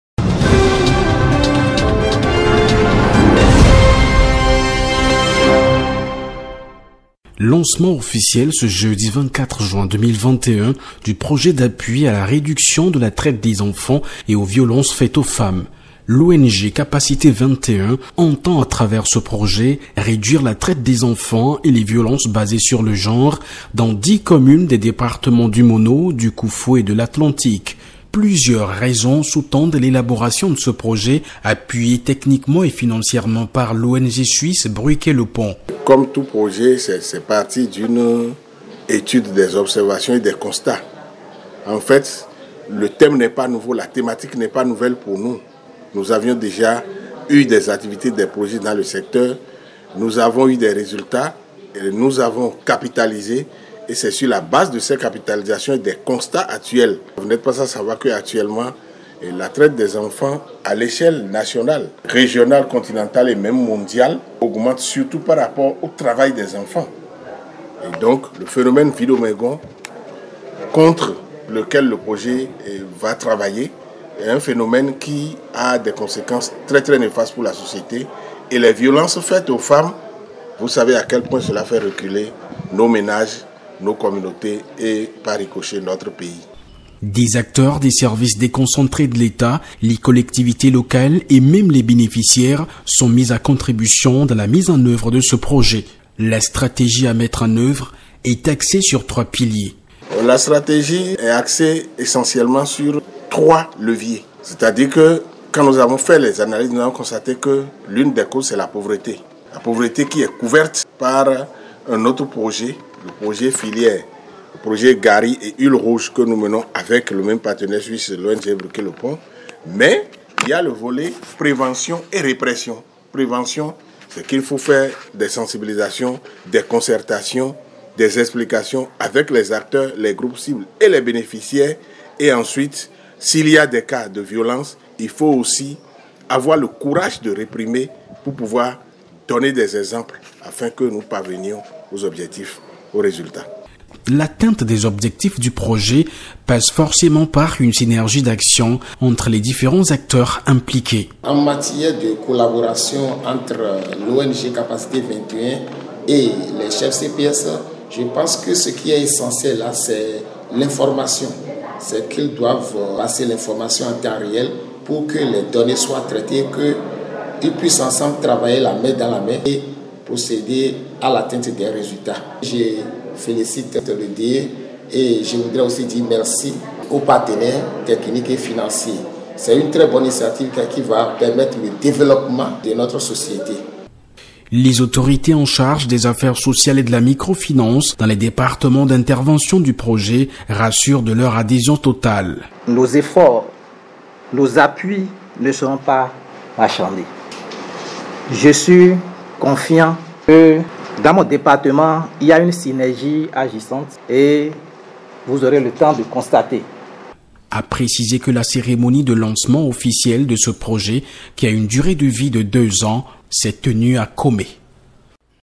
Lancement officiel du Projet " Vidomegon
L'ONG Capacités 21 poursuit la lutte contre la traite des enfants et les violences basées sur le genre